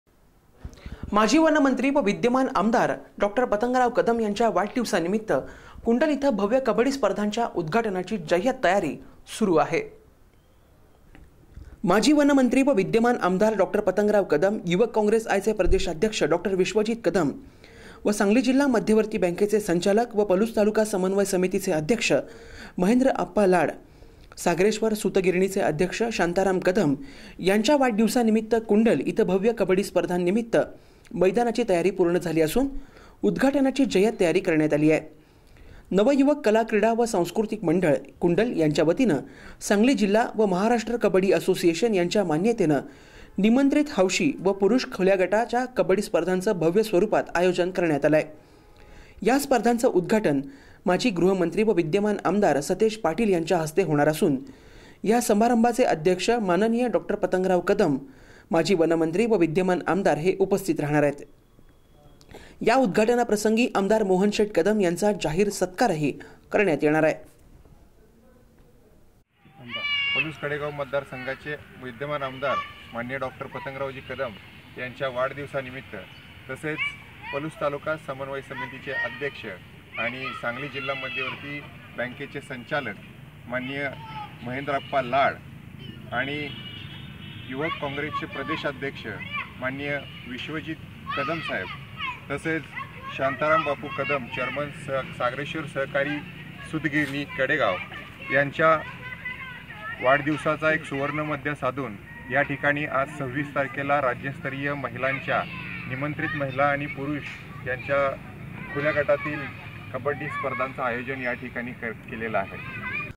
सांगली :- माजी वनमंत्री व विद्ममान आ.डॉ.पतंगराव कदम यांच्या वाढदिवसा निमित्त कुंडल येथे भव्य कबड्डी
सांगली :- माजी वनमंत्री व विद्ममान आ.डॉ.पतंगराव कदम यांच्या वाढदिवसा निमित्त कुंडल येथे भव्य कबड्डी स्पर्धेच्या उदघाटनाची जय्यत तयारी सुरु.पाहा फक्त SBN न्यूज